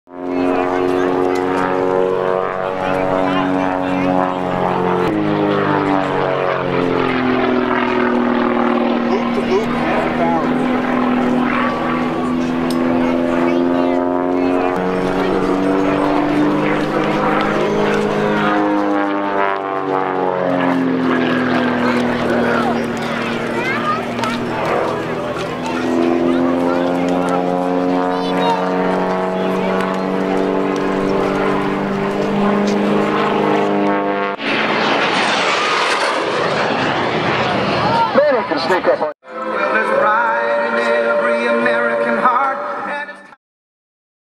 Broomfield Air Show 2009
The Colorado Sport International Air Show at the Rocky Mountain Metropolitan Airport.